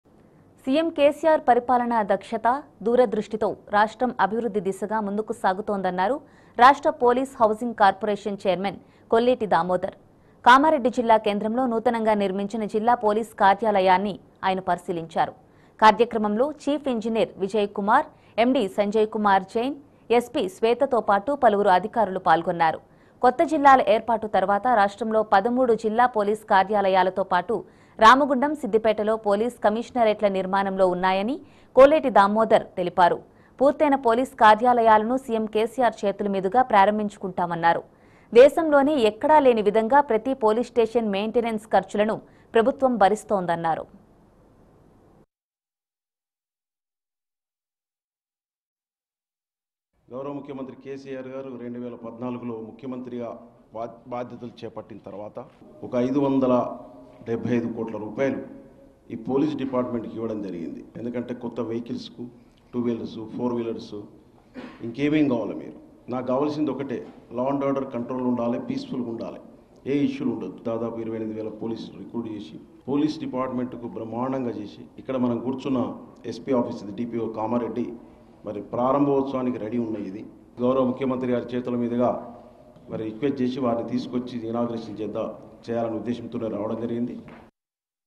కామారెడ్డిలో జిల్లా నూతన పోలీస్ కార్యాలయాన్ని పరిశీలించిన పోలీస్ హౌసింగ్ కార్పోరేషన్ చైర్మన్|T News
సీఎం కేసిఆర్ పాలనా దక్షత, దూరదృష్టి తో రాష్ట్రం అభివృద్ధి పథాన నడుస్తోందని పోలీస్ హౌసింగ్ కార్పోరేషన్ చైర్మన్ కోలేటి దామోదర్ తెలిపారు. కామారెడ్డిలో జిల్లా నూతన పోలీస్ కార్యాలయాన్ని అయన సందర్శించారు. వివిధ జిల్లాలలో పూర్తైన పోలీస్ కార్యాలయాలను సీఎం కేసిఆర్ చేతుల మీదుగా ప్రారంభించనున్నామన్నారు.